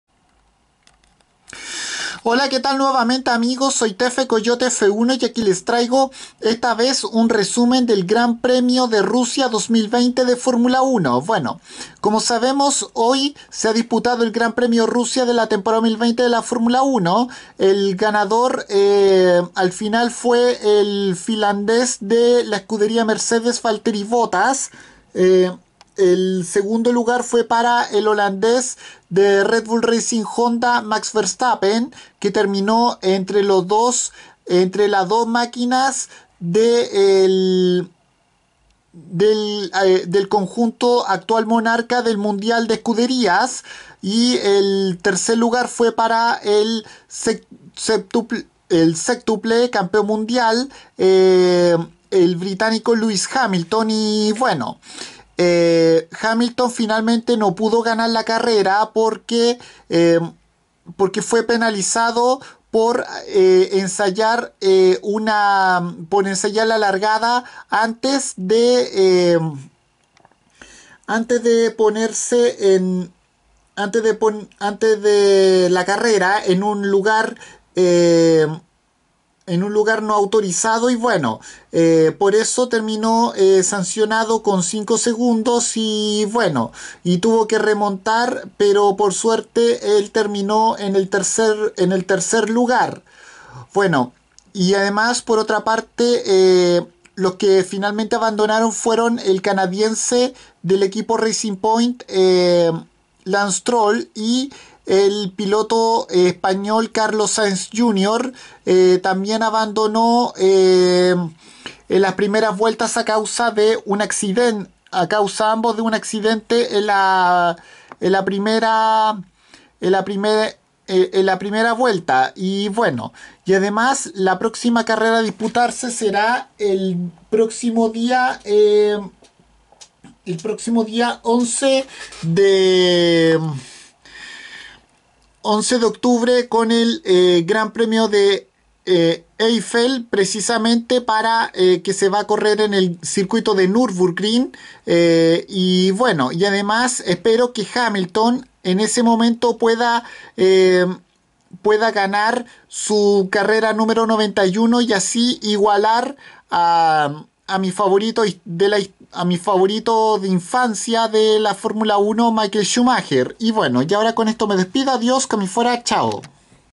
Resumen GP de Rusia 2020 de F1
Un breve resumen de lo que el Gran Premio de Rusia de la Temporada 2020 de la Fórmula 1 en la que el automovilista finlandés Valtteri Bottas obtuvo su 2da victoria del presente año seguido del holandés de Red Bull Racing Honda Max Verstappen 2do y 3ro Lewis Hamilton quien terminó en dicha posición por una sanción doble

La próxima competencia a disputarse será el Gran Premio de Eifel en el trazado normal de Alemania Nurburgring el próximo día 11 de Octubre